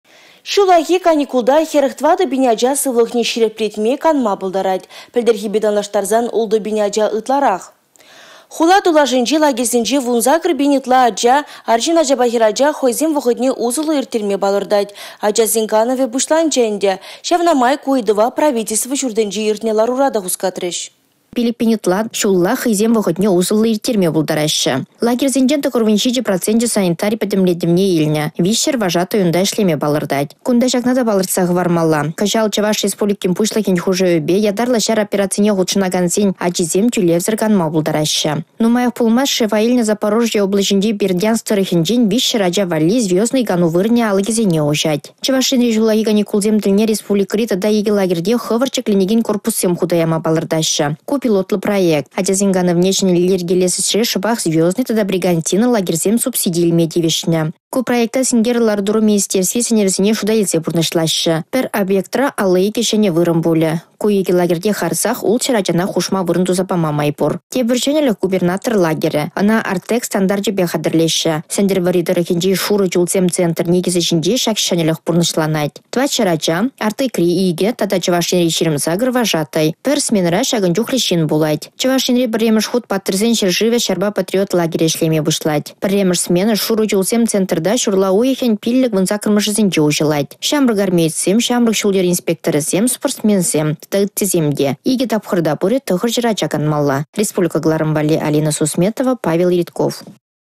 Ачасен канăвĕ пуçланчĕ
Çуллахи каникулта 44 000 ача сывлăхне çирĕплетме, канма пултарать. Пĕлтĕрхпе танлаштарсан, 6 000 ача ытларах. Хула тулашĕнчи лагерьсенче 18 000 ытла арçын ачапа хĕрача хăйсен вăхăтне усăллă ирттерме палăртать. Ачасен канăвĕ пуçланчĕ ĕнтĕ, çавна май ку ыйтава Правительство çуртĕнче иртнĕ ларура та хускатрĕç.
Хула тулашĕнчи стационарлă 16 лагерьте 18 000 ытла ачана кантарма планланă. Шкул çумĕнчи 289 кану вырăнĕ тата çакăн чухлĕ ачана йышăнать. Çавăн пекех йывăр лару-тăрăва лекнĕ çемьесенчи ачасем – вĕсем 5 000 ытла – те çулла хăйсен вăхăтне усăллă ирттерме пултараççĕ. Лагерьсенчен 97 проценчĕ санитари пĕтĕмлетĕвне илнĕ. 300 вожатый унта ĕçлеме палăртать. Кунта çакна та палăртса хăвармалла, кăçал Чăваш Республикин Пуçлăхĕн Хушăвĕпе ятарлă çар операцине хутшăнакансен ачисем тӳлевсĕр канма пултараççĕ. Нумаях пулмасть шефа илнĕ Запорожье облаçĕнчи Бердянск тăрăхĕнчен 300 ача валли «Звездный» кану вырăнĕ алăкĕсене уçать. Чăваш Енре çуллахи каникулсем тĕлне республикăри тата икĕ лагерьте хăвăрт çĕкленекен корпуссем хута яма палăртаççĕ. Ку пилотлă проект. Ачасен канăвне çĕнĕлле йĕркелес ĕçре шăпах «Звездный» тата «Бригантина» лагерьсем субсиди илме тивĕçнĕ. Ку проекта синкерлĕ лару-тăру министерстви сĕнĕвĕсене шута илсе пурнăçлаççĕ. Пĕр объектра 52 çĕнĕ вырăн пулĕ. Ку икĕ лагерьте харăсах 600 ачана хушма вырăн туса пама май пур. Тепĕр çĕнĕлĕх – «Губернатор лагерĕ». Ăна «Артек» стандарчĕпе хатĕрлеççĕ. Сĕнтĕрвăрри тăрăхĕнчи «Шурă чулсем» центр никĕсĕ çинче çак çĕнĕлĕх пурнăçланать. 400 ача, «Артекри» икĕ тата Чăваш Енри 28 вожатăй – пĕр сменăра çакăн чухлĕ çын пулать. Чăваш Енре пĕрремĕш хут «Паттăрсен çĕршывĕ» çарпа патриот лагерĕ ĕçлеме пуçлать. Пĕрремĕш смена «Шурă чулсем» центрта çурла уйăхĕн 5-18-мĕшĕнче уçăлать. Çамрăк армеецсем, çамрăк çул-йер инспекторĕсем, спортсменсем – икĕ тапхăрта пурĕ 900 ача канмалла.